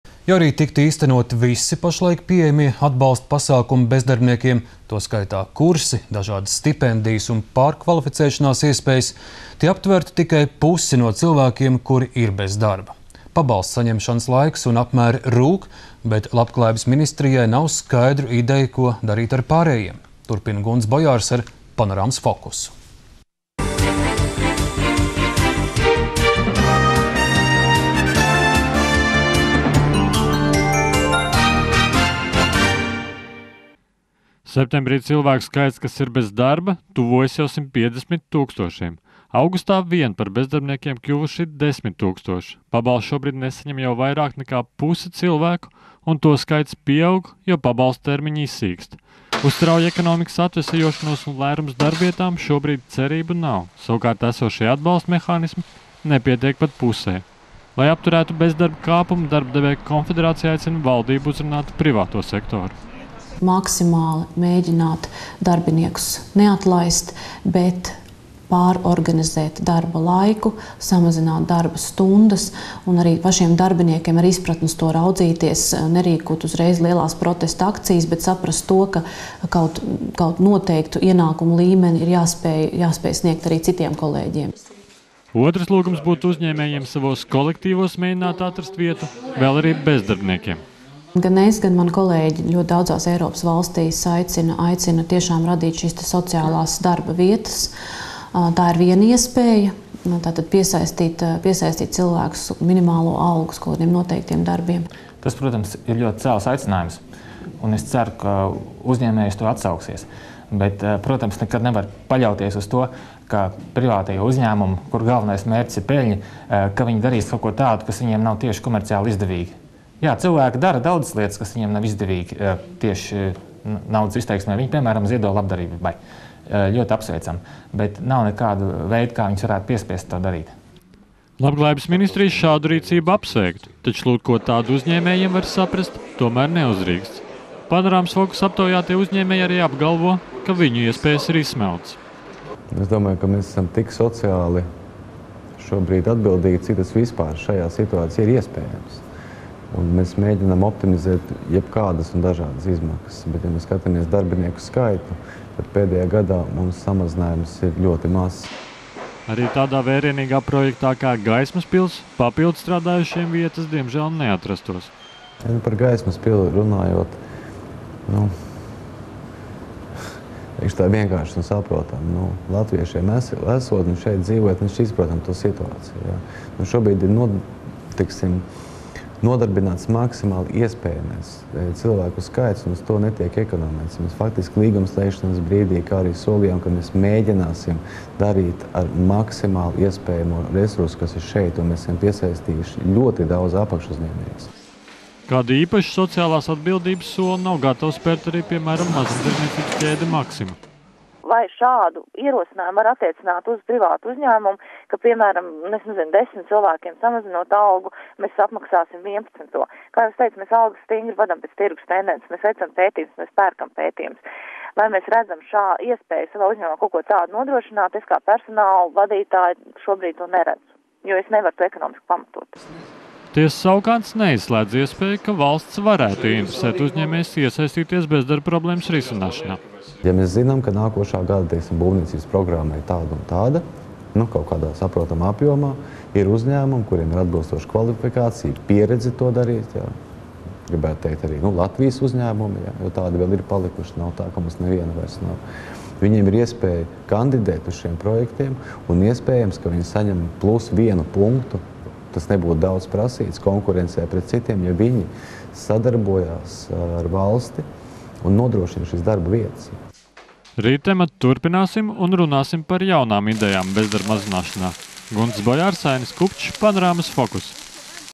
LM nezin, ko darīt ar bezdarbniekiem
Ja arī tiktu īstenoti visi pašlaik pieejamie atbalsta mehānismi bezdarbniekiem, to skaitā kursi, dažādas stipendijas un pārkvalificēšanās iespējas, tie aptvertu tikai pusi no cilvēkiem, kuri ir bez darba. Pabalstu saņemšanas laiks un apmēri rūk, bet Labklājības ministrijai nav stratēģisku ideju, ko darīt ar pārējiem.